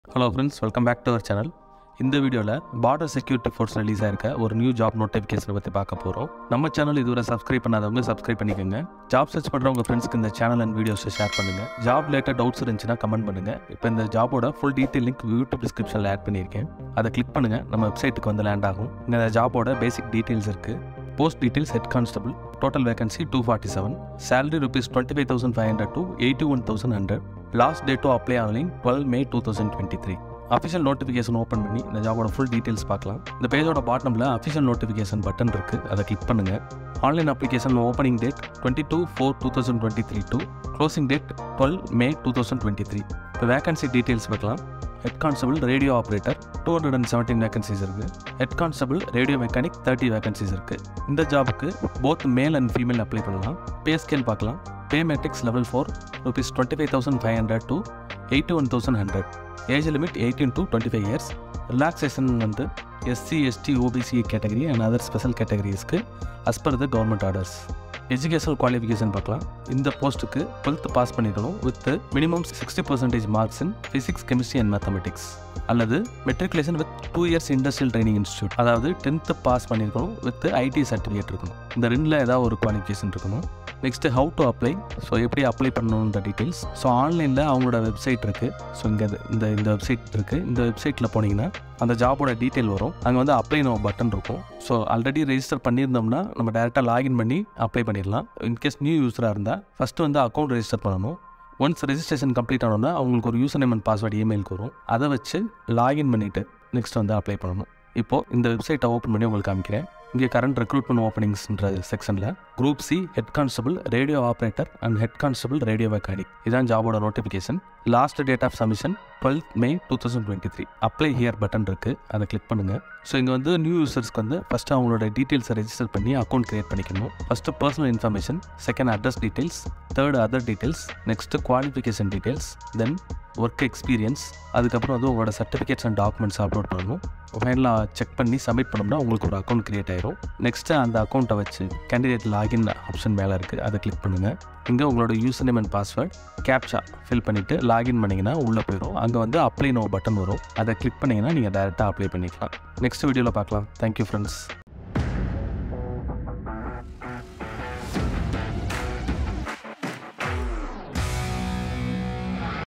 BSF Head Constable RO/RM Recruitment 2023 | Full Details Step by Step in Tamil | 247 Posts
FOLLOW FOR MORE JOBS UPDATES